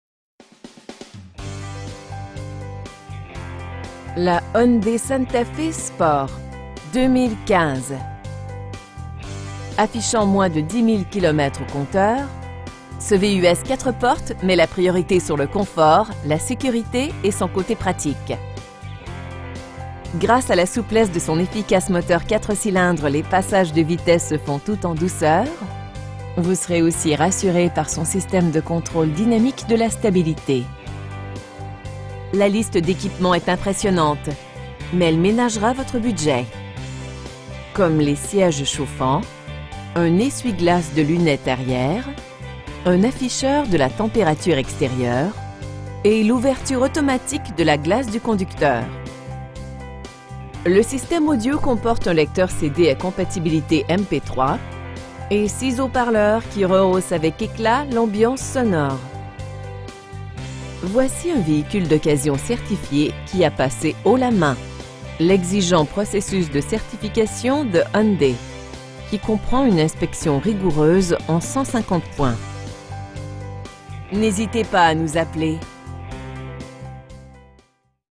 2015 Hyundai Santa Fe Sport 2.4 FWD Certified intere
Hyundai Gabriel
7020 Henri Bourassa Est

Le Hyundai Santa Fe Sport 2015. Affichant moins de 10 000 km au compteur, ce VUS 4 portes met la priorit�� sur le confort, la s��curit�� et son c��t�� pratique. Gr��ce �� la souplesse de son efficace moteur 4 cylindres, les passages de vitesses se font tout en douceur, vous serez aussi rassur�� par son syst��me du contr��le dynamique de la stabilit��. La liste d'��quipement est impressionnante, mais elle m��nagera votre budget. Comme les si��ges chauffants, un essuie-glace de lunette arri��re, un compte-tours, un ordinateur de bord, un afficheur de la temp��rature ext��rieure, une banquette arri��re �� dossiers rabattables s��par��s, galerie de toit, et l'ouverture automatique de la glace du conducteur. Ses attrayantes roues en alliage s'harmonisent aux formes des ailes qui accentuent le caract��re sportif du v��hicule. Le syst��me audio comporte un lecteur CD �� compatibilit�� MP3, commandes audio sur le volant, et 6 haut-parleurs qui rehaussent avec ��clat l'ambiance sonore. Les coussins de s��curit�� gonflables lat��raux et autres dispositifs de s��curt�� vous procurent une protection de haut niveau. Voici un v��hicule d'occasion certifi�� qui a pass�� haut la main l'exigent processus de certification de Hyundai, qui comprend une inspection rigoureuse en 150 points! Notre fiert�� se manifeste �� travers l'excellent service �� la client��le que nous offrons. N'h��sitez pas �� nous appeler.